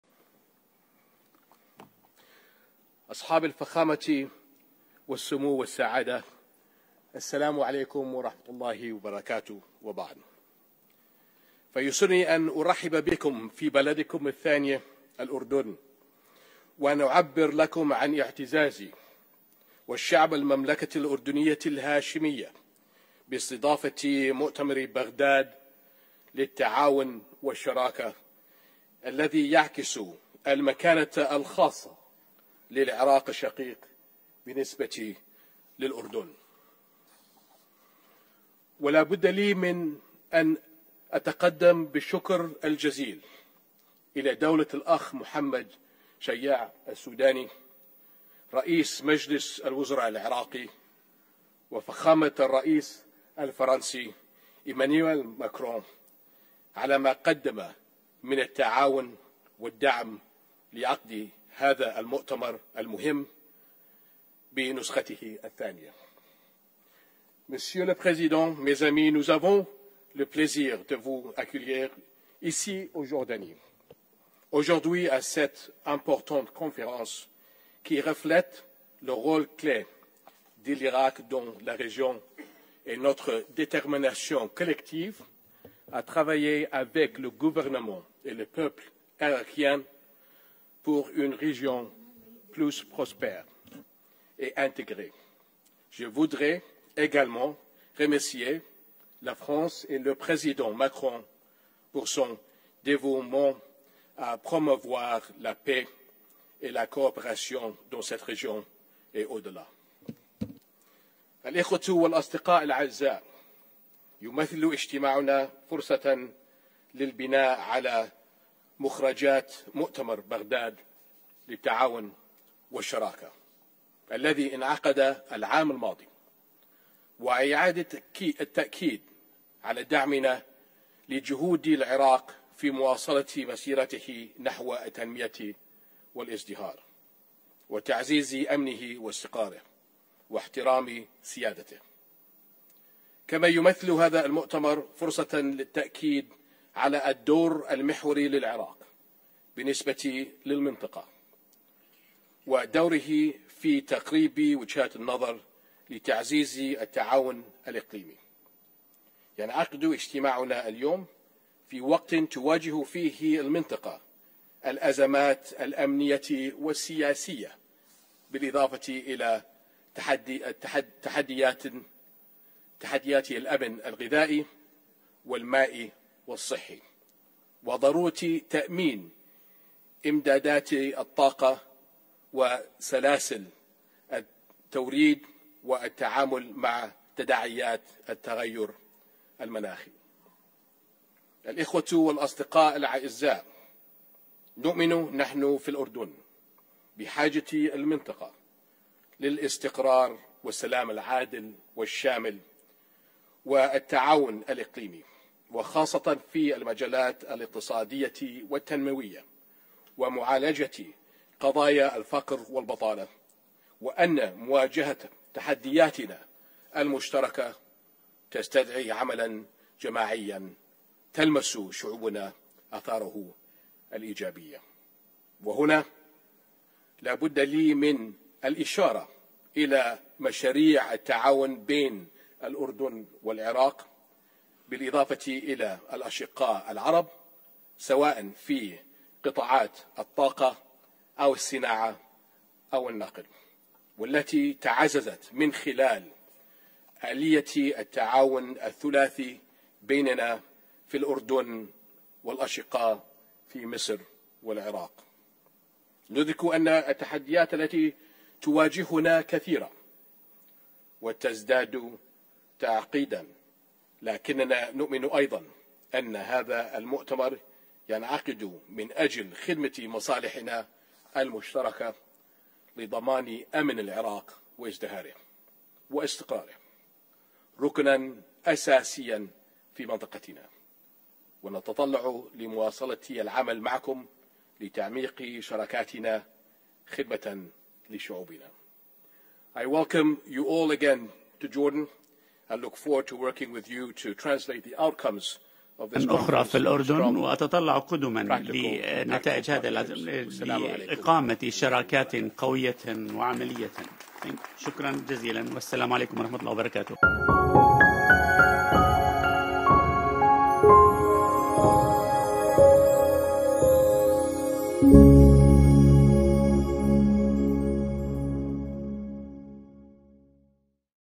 الملك يلقي كلمة في افتتاح مؤتمر بغداد للتعاون والشراكة في دورته الثانية باستضافة الأردن
الملك يلقي كلمة في افتتاح مؤتمر بغداد للتعاون والشراكة في دورته الثانية باستضافة الأردن، في منطقة البحر الميت